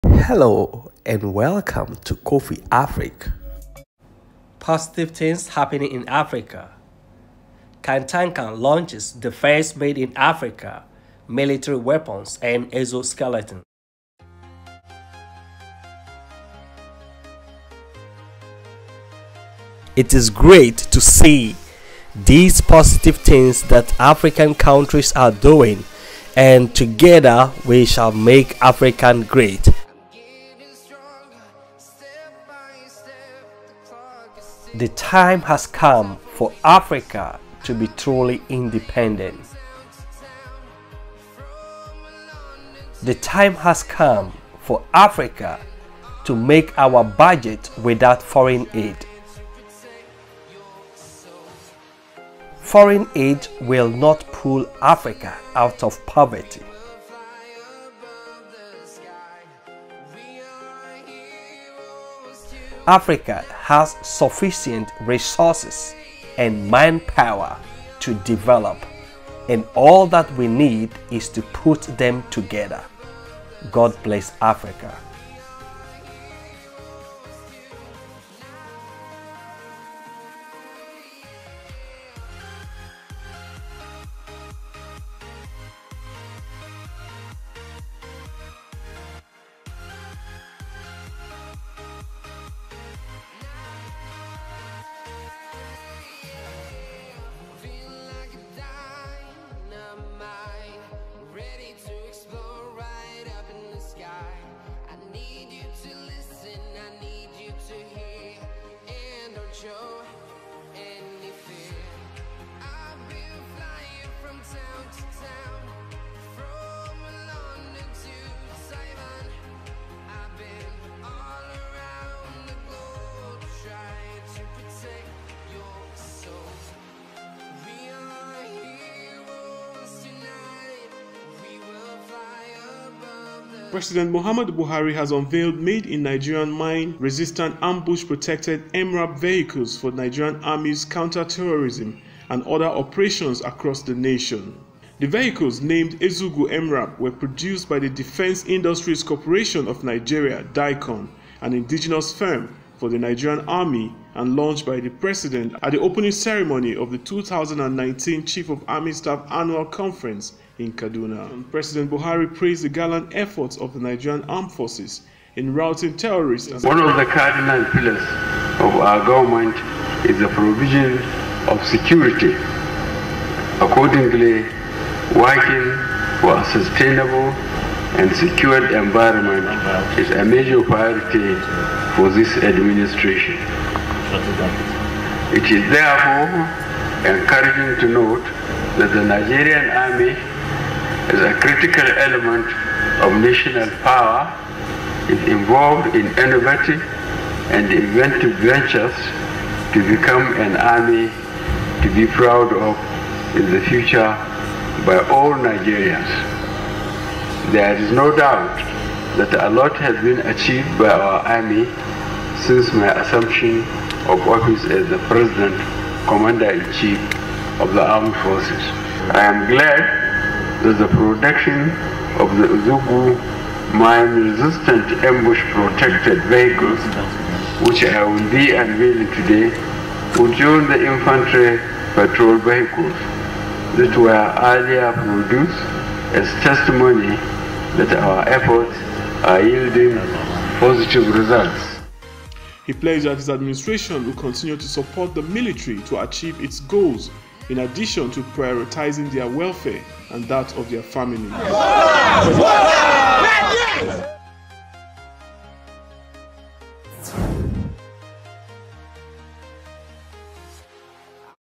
Ghana - Nigeria Cars
Ghana and Nigeria have taken the lead in manufacturing armoured vehicles for their military. This is in the right direction. As Africans we need to help ourselves. We are humans with sharp brains and the time is ripe for us to let the world know that Africans are capable of managing our own affairs.